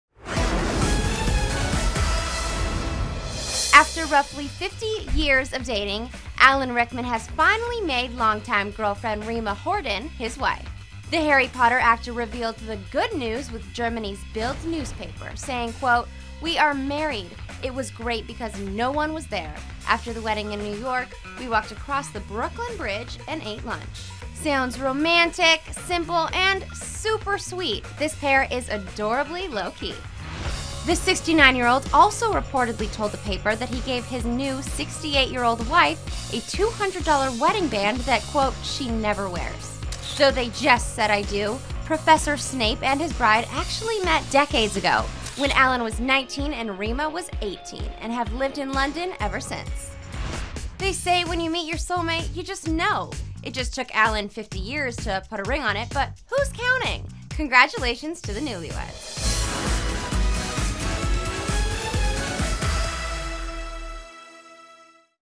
Alan Rickman Marries Longtime Love Rima Horton After Nearly 50 Years!
Professor Snape took nearly half a century to put a ring on it!